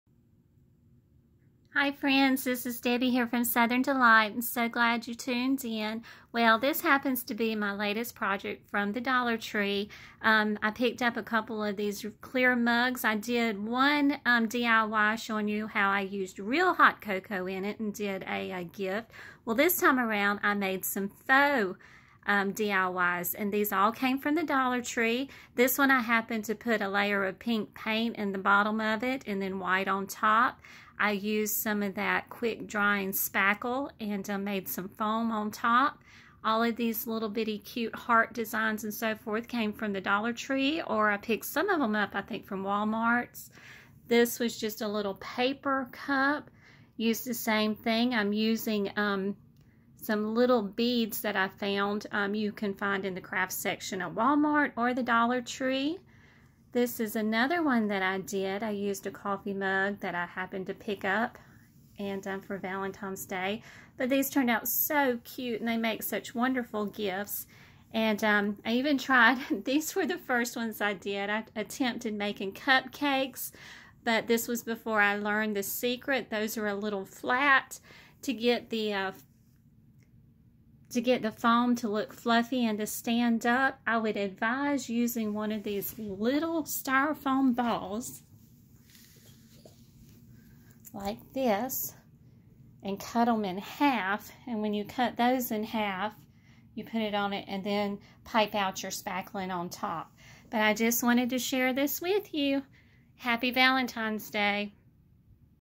Dollar Tree Valentine FAUX COFFEE MUG
This is a short on a few faux coffee mugs I made. These are great for valentines day gifts. They also make great props for pictures and videos.